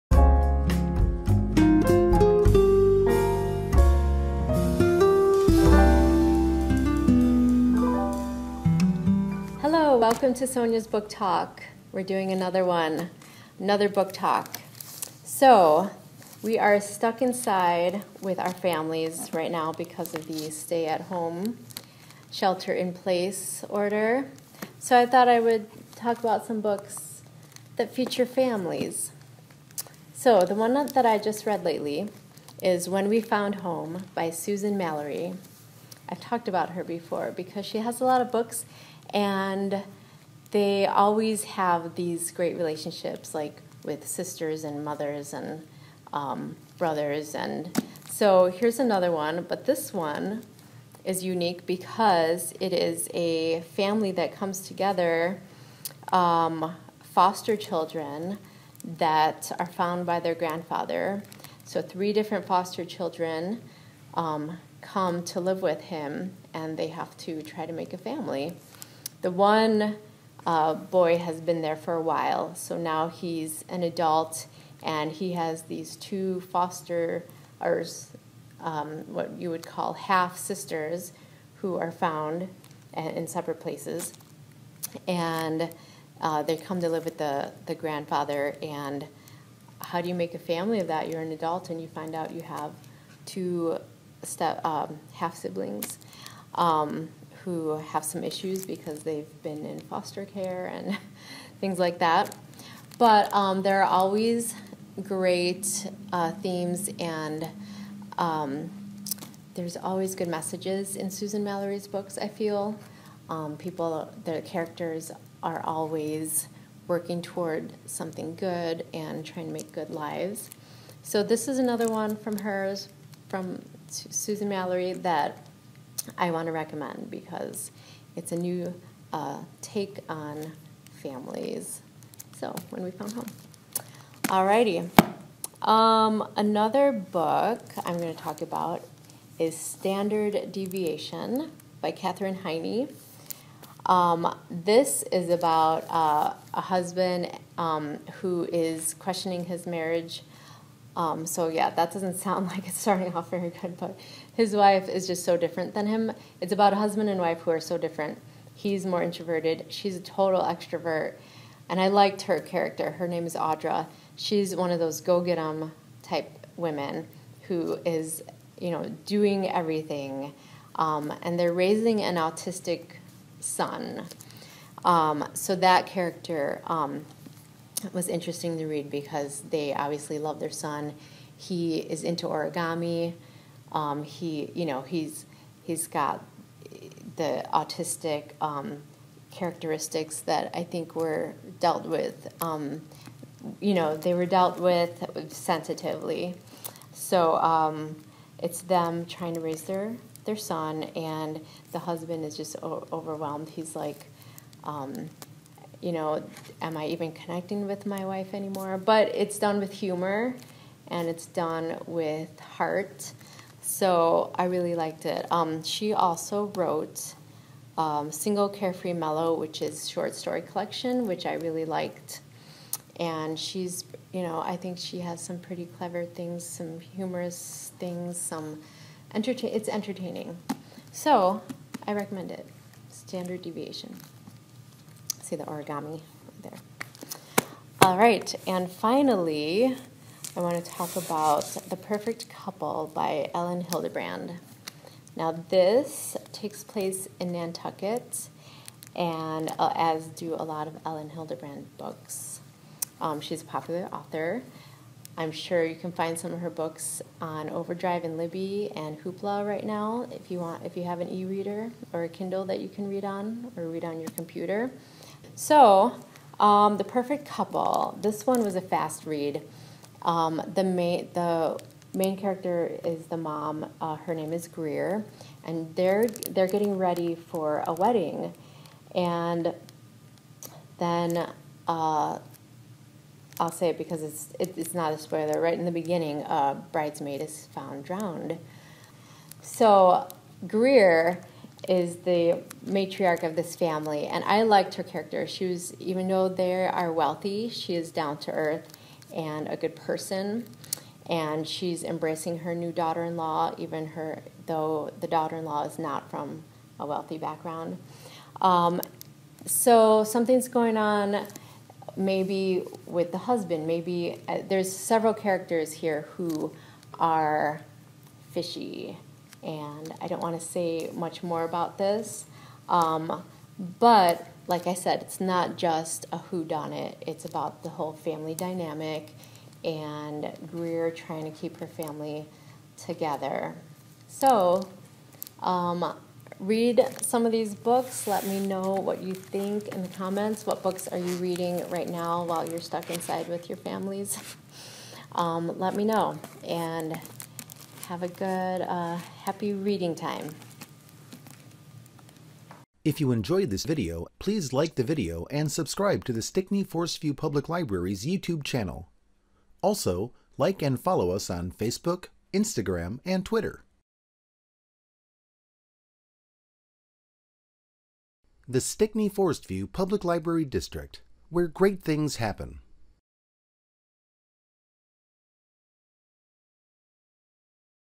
Sonia's Book Talk: All in the Family
Did you know that Sonia has made 40 of her Book Talk Videos? Join us tonight for the premiere of Sonia's Book Talk episode 41, All in the Family, where Sonia discusses three books which focus on families.

Click the links in the comments below to see the books in the Library's online catalog where you can check availability and place holds. The titles are possibly also available in other formats, such as audio books, large print, or e-books.